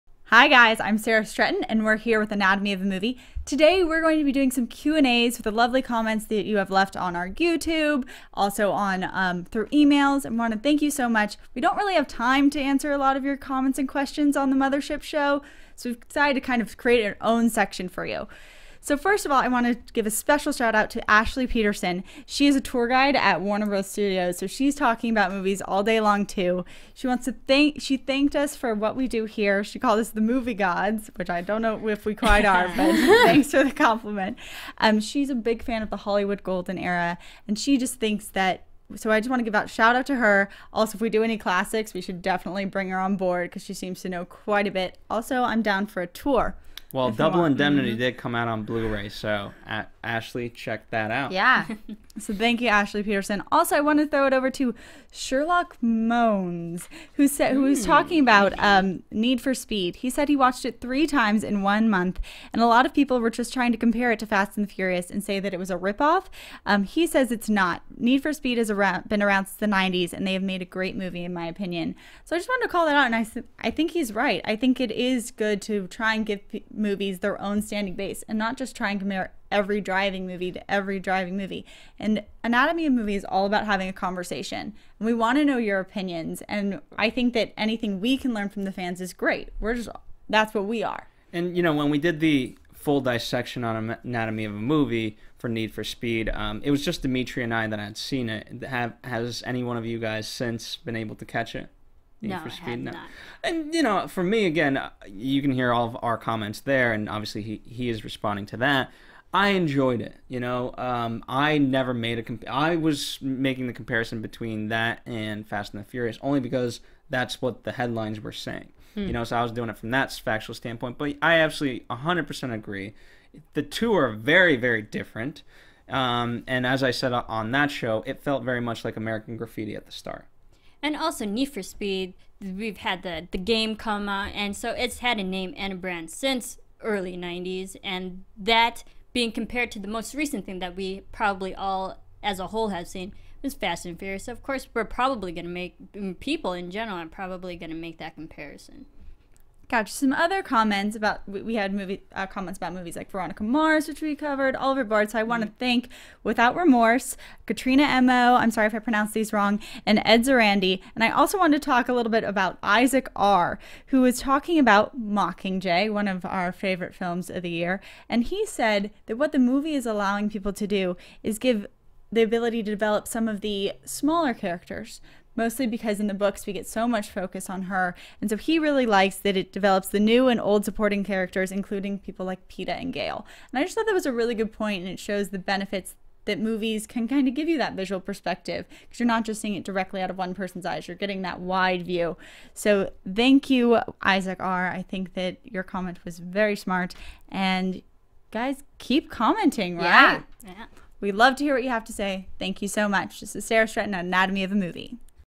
Fan Q & A for April 16, 2014 | Anatomy of a Movie
Anatomy of a Movie hosts Sara Stretton, Chloe West, Marisa Serafini and Phil Svitek answer some fan mail and give shout outs to top commentators.